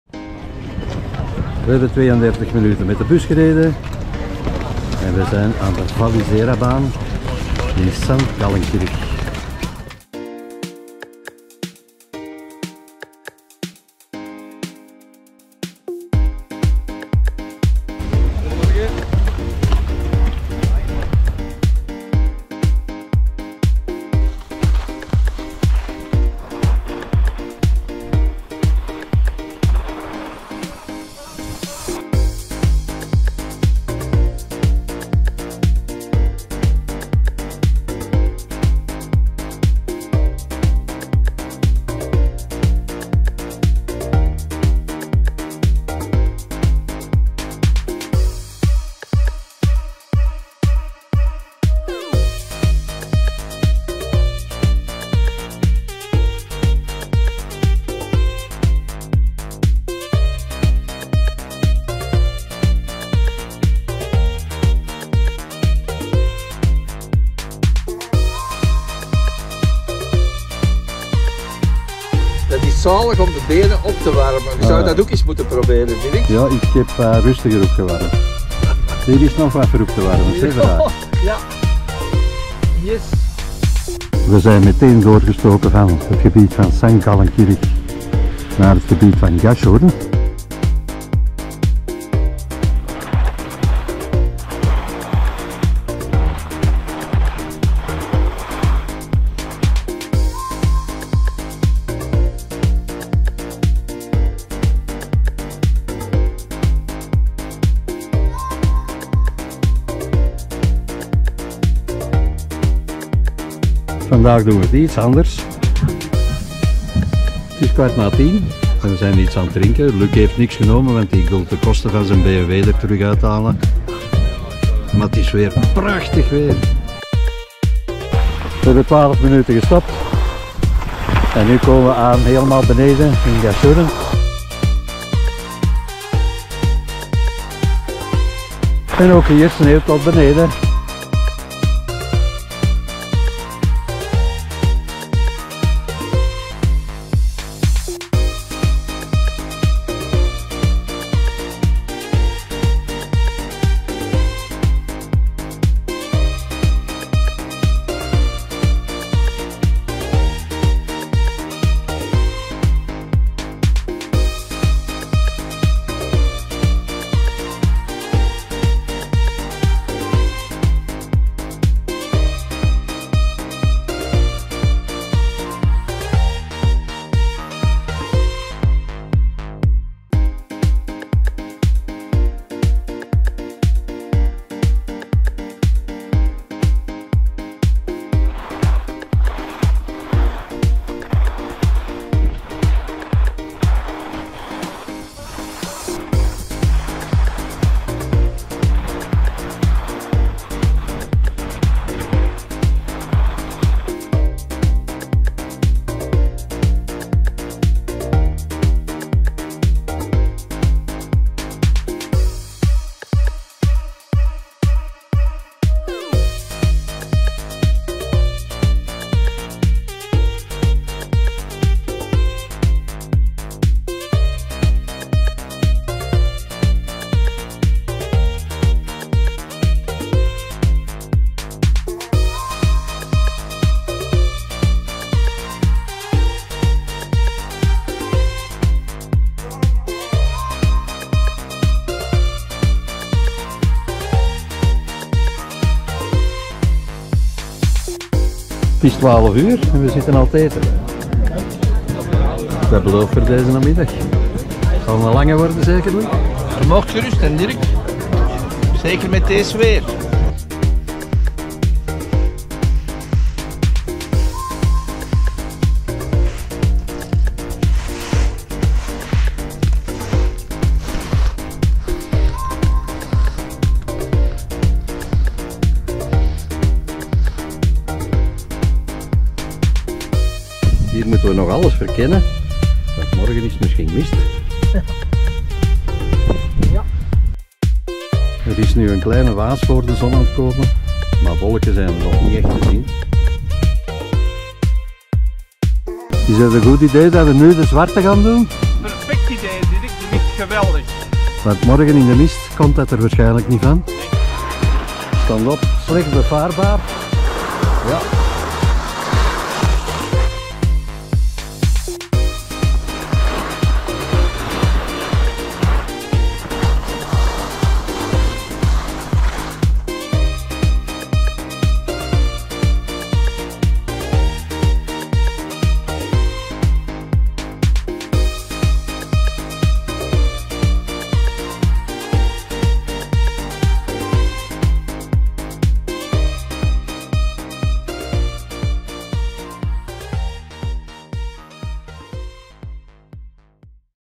Skiën,Oostenrijk,Vorarlberg,Montafon,Silvretta Montafon,Sankt Gallenkirch,Gaschurn,deel 2
The second day we drive by bus to Sankt Gallenkirch where we take the cable car. We immediately cross to the area of Gaschurn, where we explore all the slopes. After lunch we will continue to explore the area in Sankt Gallenkirch. There is still something left for tomorrow!

De tweede dag van ons skiverlof rijden we met de bus naar Sankt Gallenkirch waar we de kabelbaan nemen. We steken meteen door naar het gebied van Gaschurn, waar we alle pistes verkennen. Na de lunch gaan we het gebied in Sankt Gallenkirch verder verkennen. Er blijft nog wat over voor morgen!